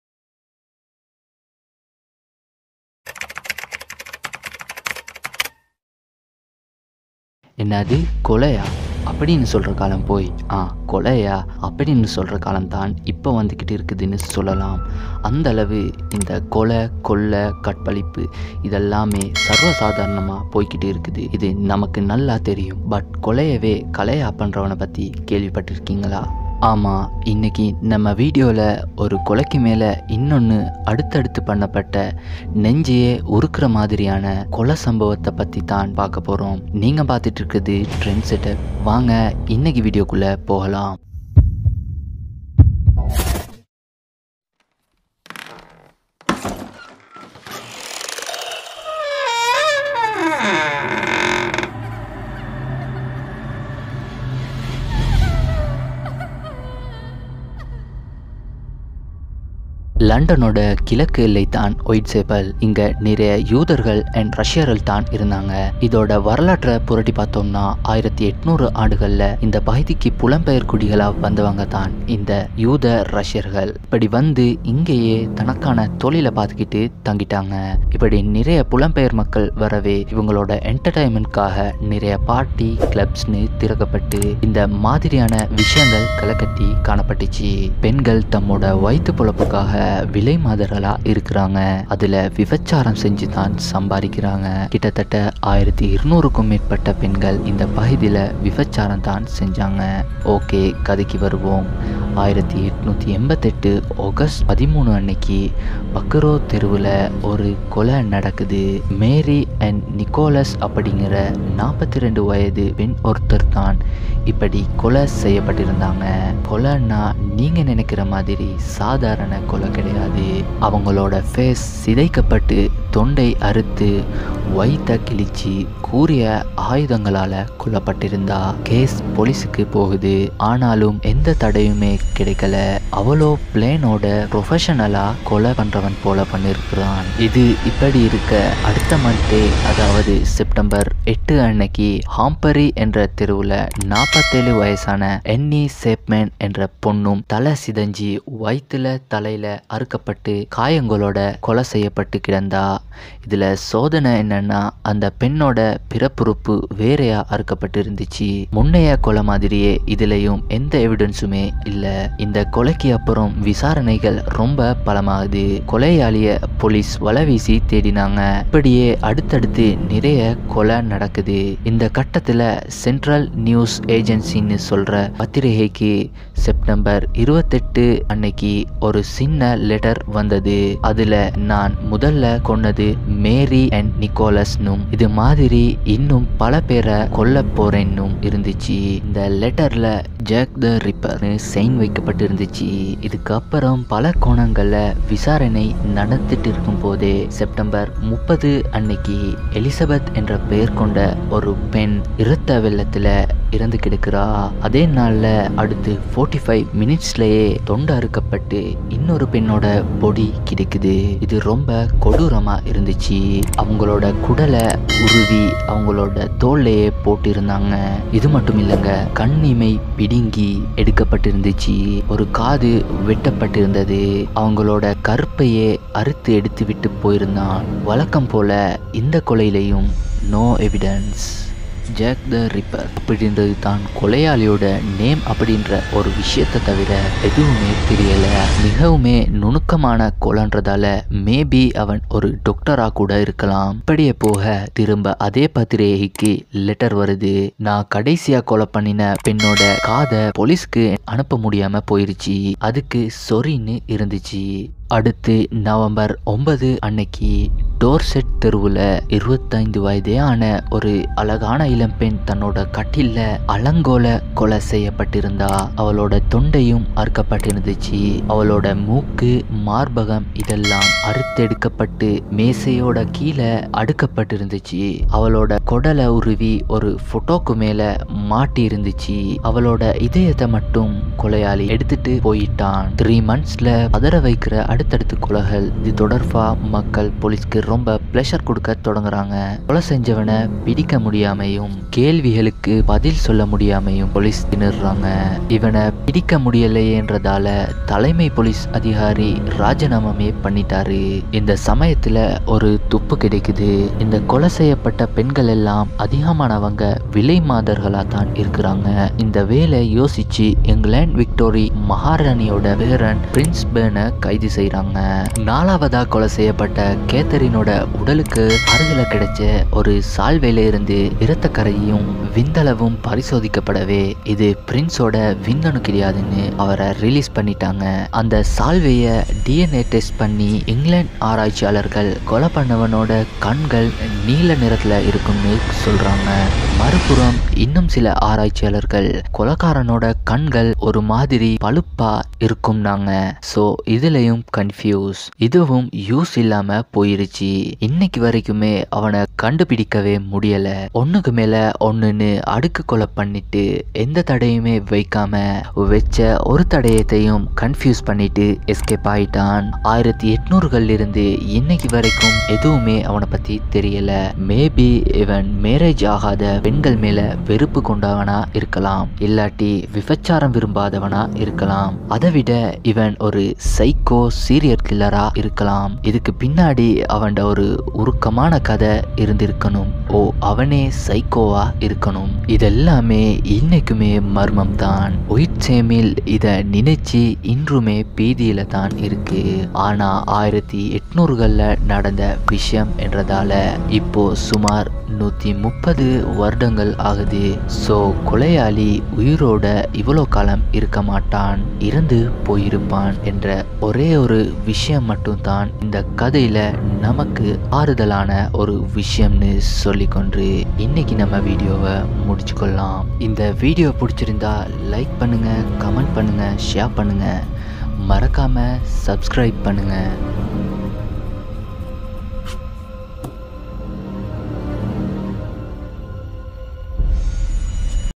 Jack The Ripper/கொலையவெ கலையா பண்ணவன் 🤔/ Trend setup
🅾️ Sorry if there were any problems or errors in this video
Jack The Ripper Real story

😲Jack the Ripper was an unidentified serial killer active in the largely impoverished areas in and around the Whitechapel district of London in 1888. In both the criminal case files and contemporary journalistic accounts, the killer was called the Whitechapel Murderer and Leather Apron

☞︎︎︎ The story is told as concisely as possible

💓Tʜᴀɴᴋs ғᴏʀ ᴡᴀᴛᴄʜɪɴɢ💓